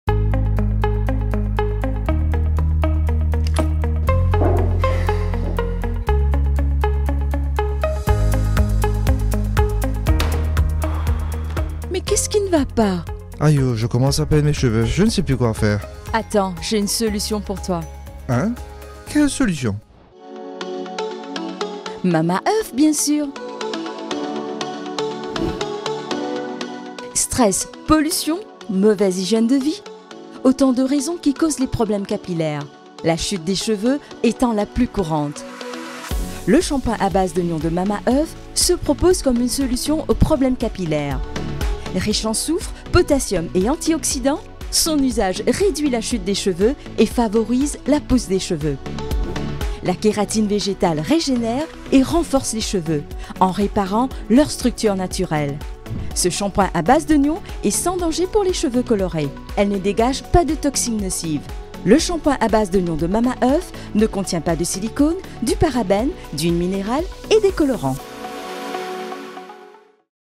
[Contenu sponsorisé] Le shampooing à base d’oignon de Mamaearth
[Contenu sponsorisé] Le shampooing à base d’oignon de Mamaearth se propose comme une solution au problème de chute de cheveux

Stress, pollution, mauvaise hygiène de vie… Autant de raisons qui causent les problèmes capillaires, la chute des cheveux étant la plus courante.

Le shampooing à base d’oignon de Mamaearth se propose comme une solution de lutte à ces problèmes. Riche en soufre, potassium et antioxydant, son usage réduit la chute des cheveux et favorise la pousse des cheveux.

La kératine végétale régénère et renforce les cheveux, en réparant leur structure naturelle.

Ce shampooing à base d'oignon est sans danger pour les cheveux colorés. Elle ne dégage pas des toxines nocives. Il ne contient pas de silicones, du parabène, d’huiles minérales et des colorants.